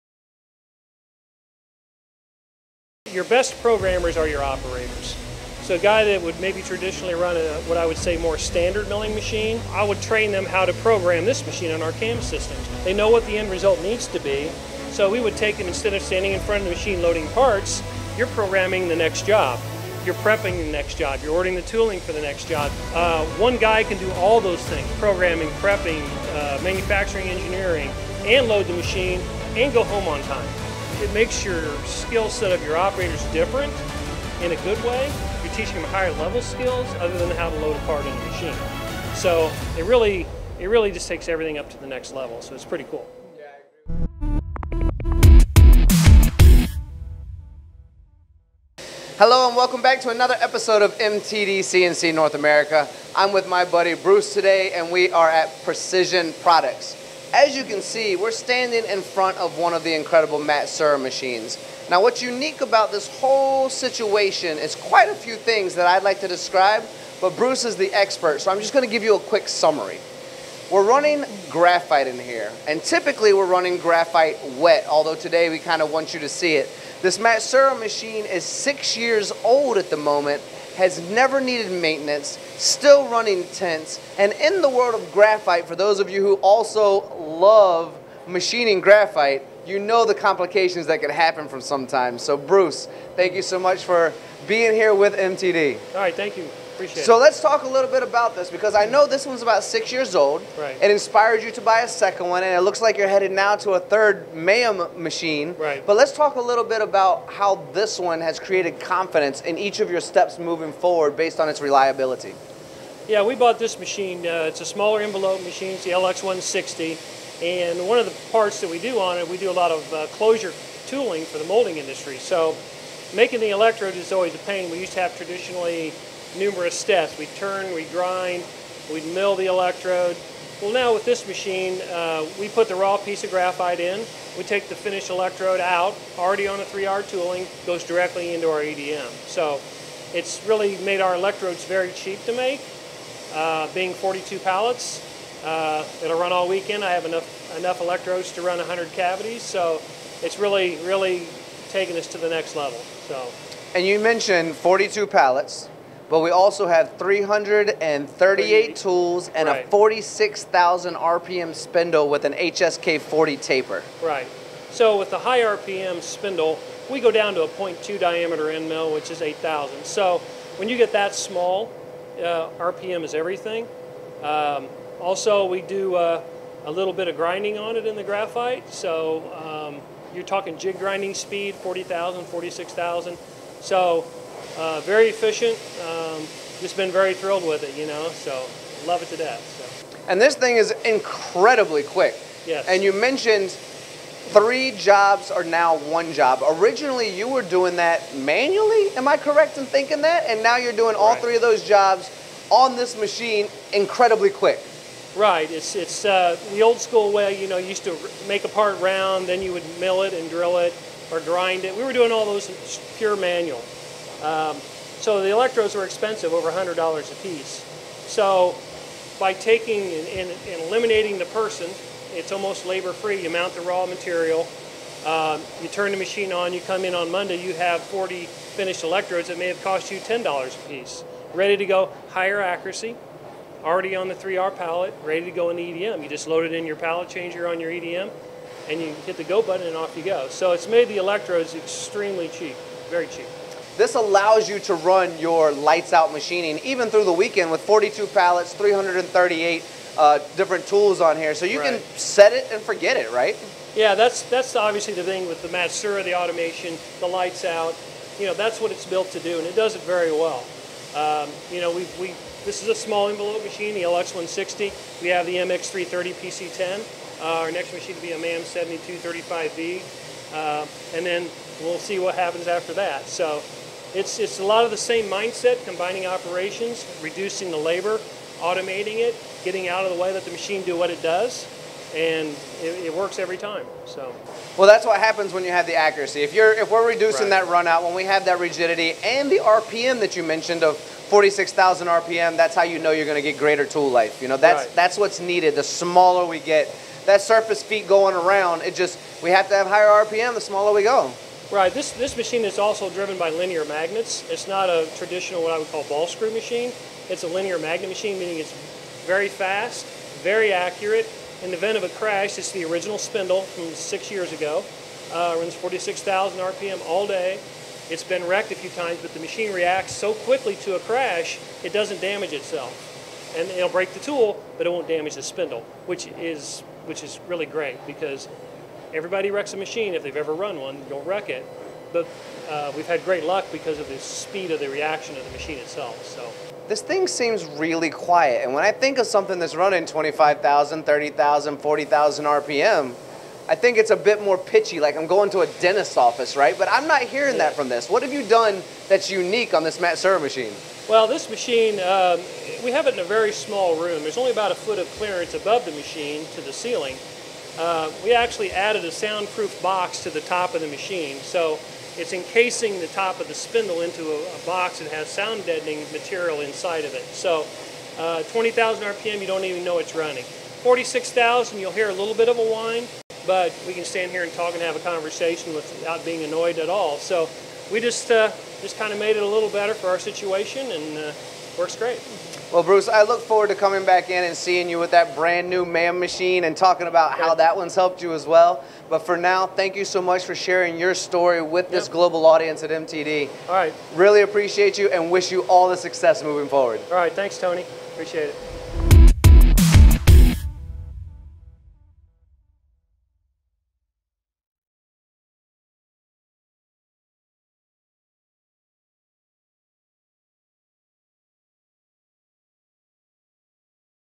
LX-160: Electrode cost $100 to $10 | 電極製作コストを90%削減
With powerful lights-put operation with 42 pallets and 338 tools on the LX-160, Precision Products has achieved the significant cost down on their electrode manufacturing by 90%!
The finished electrodes are already on the 3R pallets, so they are ready to go to EDM directly.

42枚のパレットと338本の工具によるLX-160の無人運転により、Precision Products社は電極製作コストを90%削減しました。
完成した電極は3Rのホルダーの載っているため、そのままEDM機に取り付けることが出来ます。